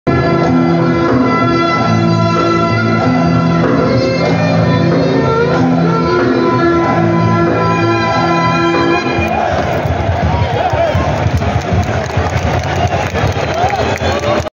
ERZİNCANSPOR - URFA FİNAL MAÇI.. MUHTEŞEM ERZİNCAN TARAFTARI
Anagold 24 Erzincanspor Tff 2. lig play off finali 07 Haziran 2023